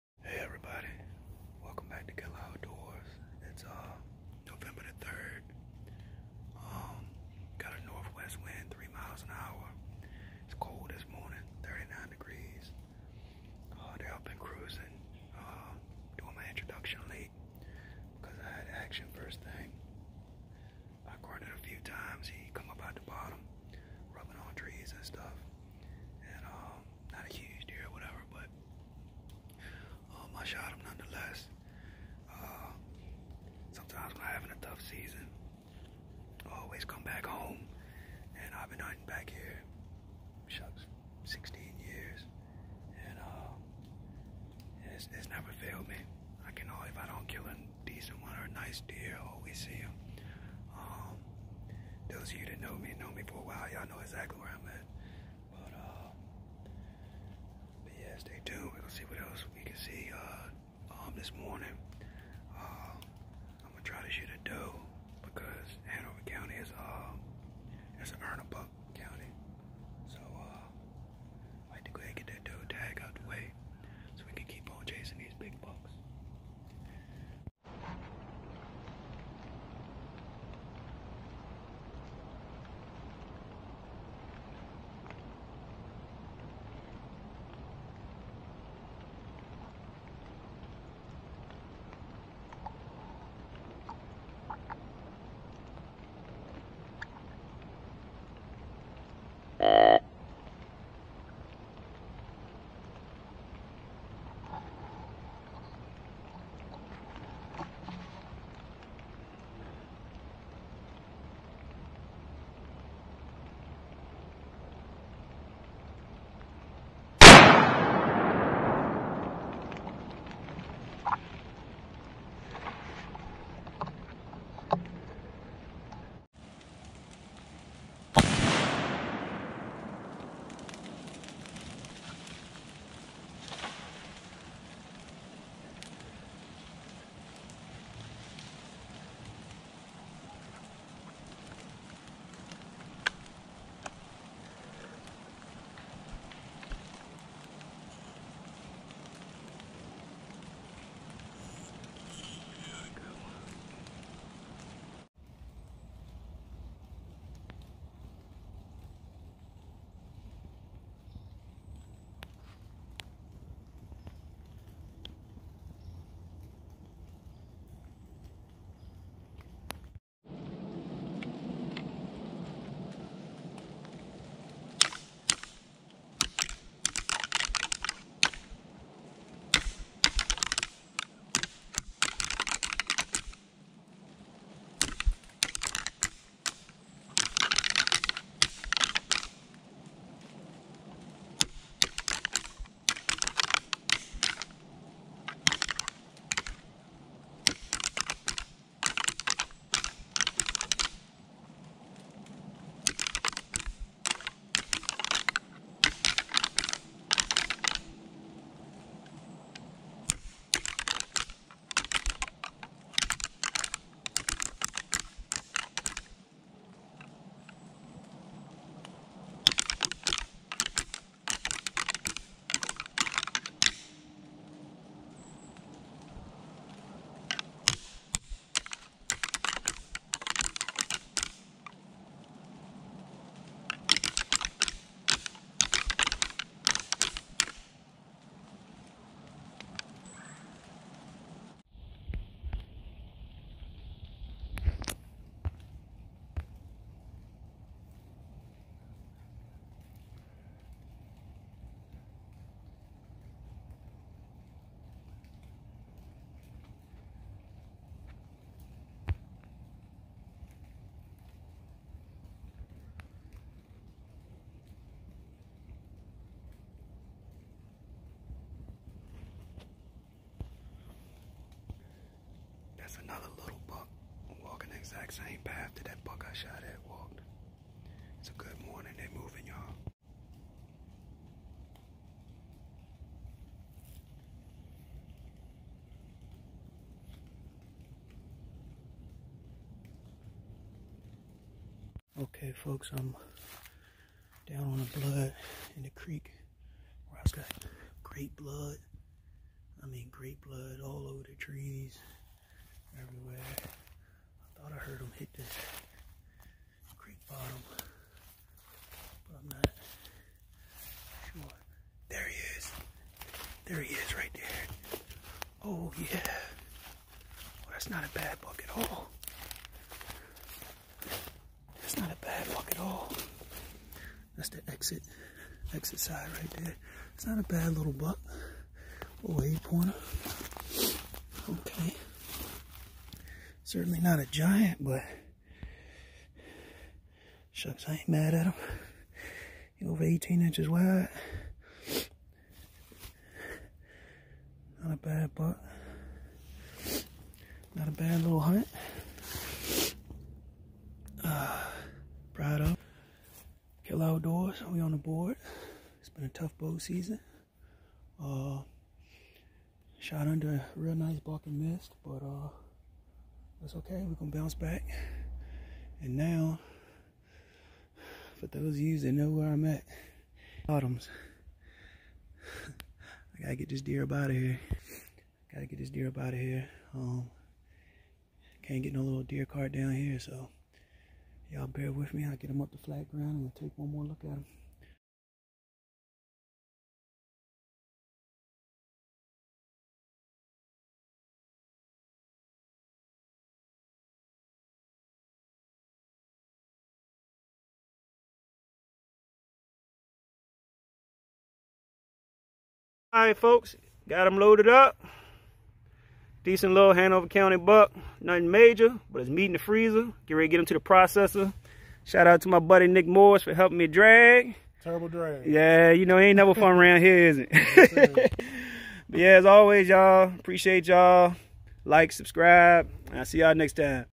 Big Buck Down!!! 2021 Virginia Muzzleloader Hunt
Finally connected on a shooter buck! Been a tough bow season but things are starting to look up!